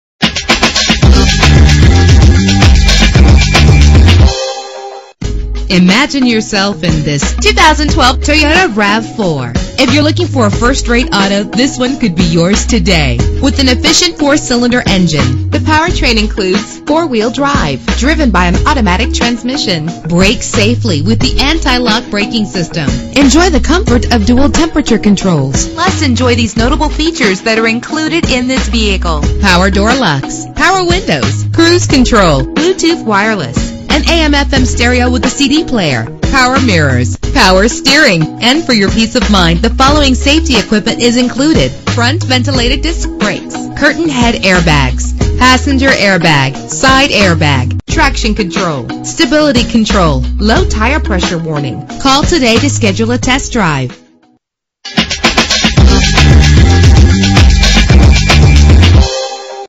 2012 Toyota RAV4 - Springfield OH
Year: 2012
Make: Toyota
Model: RAV4
Trim: Limited
Engine: 2.5 liter inline 4 cylinder 16 valve
Transmission: 4-Speed Automatic
Color: Blizzard Pearl
Mileage: 0
Address:
1501 Hillcrest Ave